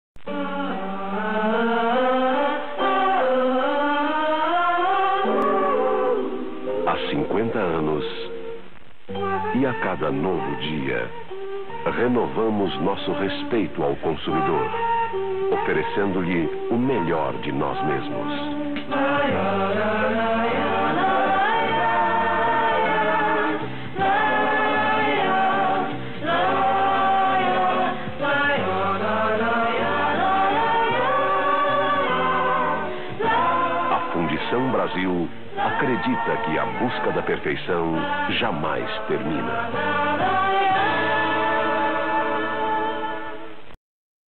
Fogão Continental 2001 Comercial 1976
Comercial raríssimo e muito bem criado para a apresentação da linha de fogões Continental 2001. Hoje esse comercial não teria o mesmo glamour por questões óbvias.